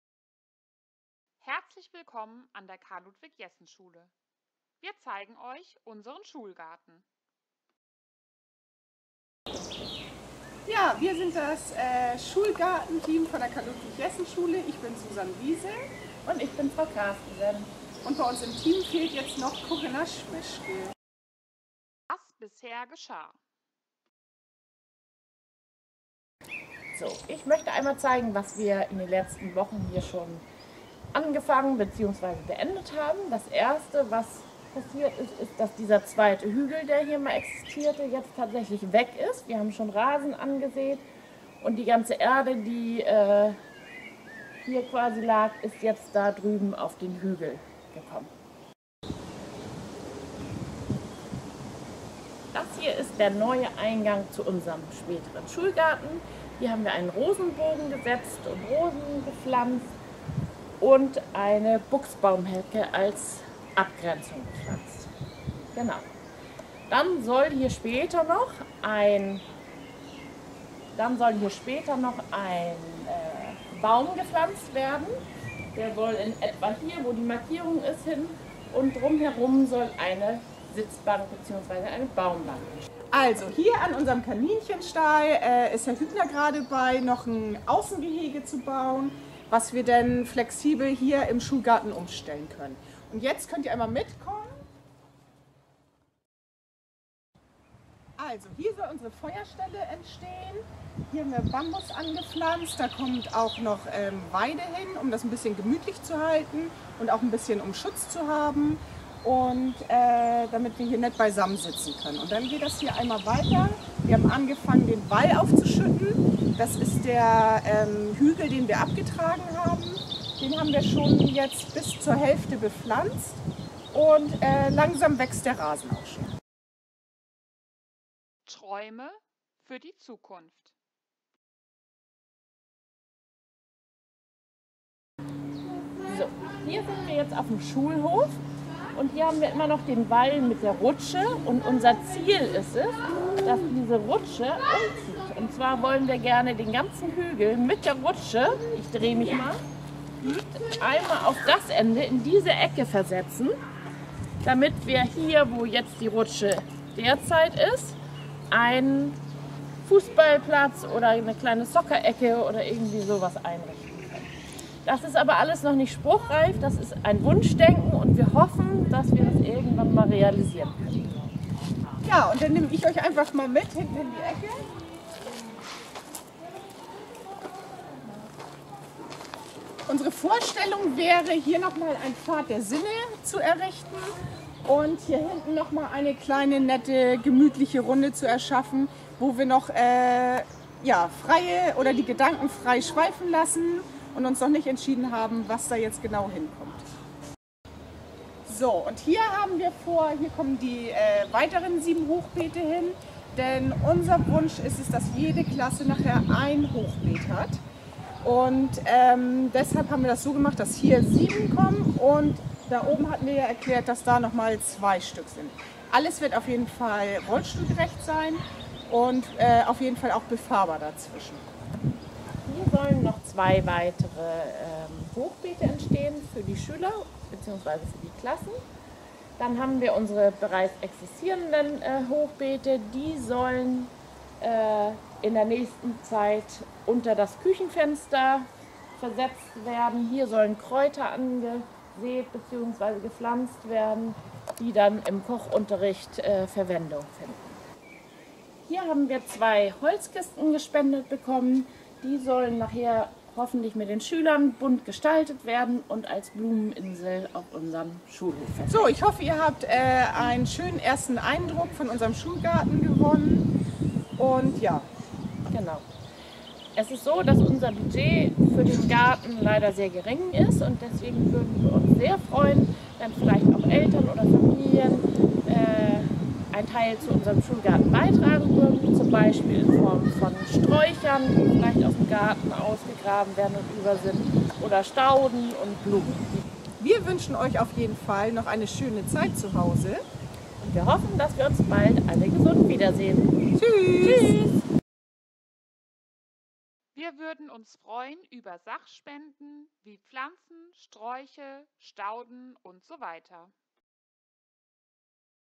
Schulgartenfilm
Wir zeigen euch, was in unserem Schulgarten so passiert.
Wir freuen uns über Sachspenden wie Pflanzen, Sträucher, Stauden und so weiter. :-)